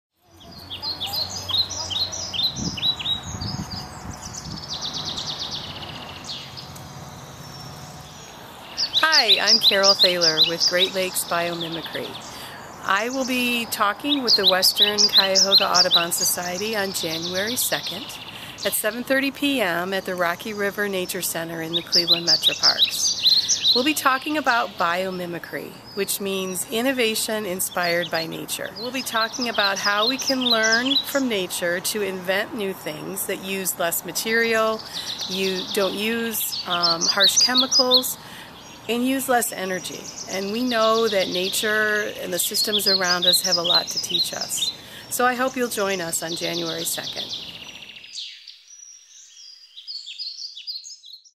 Great Lakes Biomimicry: Learning From (not just about) Nature
Western Cuyahoga Audubon Speaker Series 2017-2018

Program: Great Lakes Biomimicry: Learning From (not just about) Nature
Presenter: Carol Thaler, Director of Outreach and Administration, Great Lakes Biomimicry
Date: Tuesday, January 2, 2018, at 7:30 p.m.
Location: Rocky River Nature Center, 24000 Valley Pkwy, North Olmsted, OH 44070

Description: “Thinking of nature as a teacher rather than a warehouse is a powerful act.” This statement, by author Janine Benyus, has sparked people to learn more about biomimicry. Biomimicry, or innovation inspired by nature, has taken root here in Northeast Ohio. Nature, through its 3.8 billion year history of Research and Development, has evolved solutions to many of the problems that humans try to solve. By emulating nature’s time-tested patterns and strategies, humans can innovate to create sustainable solutions. Join Carol Thaler, Director of Outreach and Administration for Great Lakes Biomimicry, as she shares the basics of biomimicry, examples of how it is being used, and how some species hold great insights to solve our problems, in her presentation “Great Lakes Biomimicry: Learning From (not just about) Nature.”

Recorded at Lake Erie Nature and Science Center, 28728 Wolf Rd, Bay Village, OH 44140